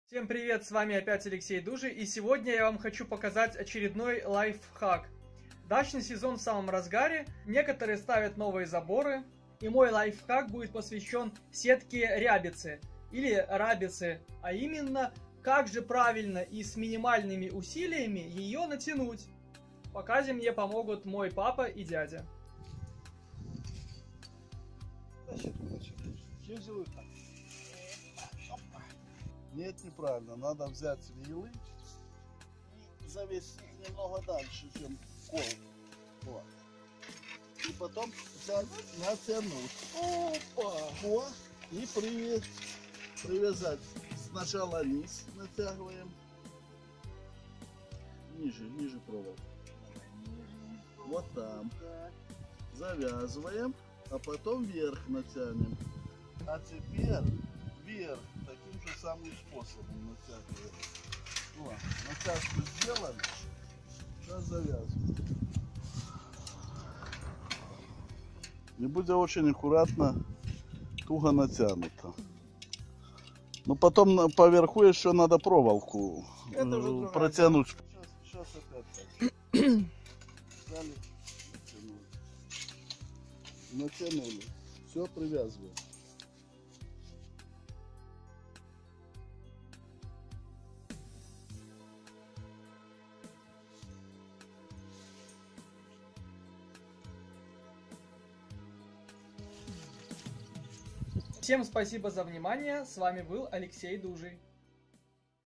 Забор из Сетки Рабица. Как НАТЯНУТЬ Сетку Рабица. ЛАЙФХАК. How INSTALL chain link FENCE
Как правильно натянуть сетку-рабицу с минимальным усилием - смотрите в видео.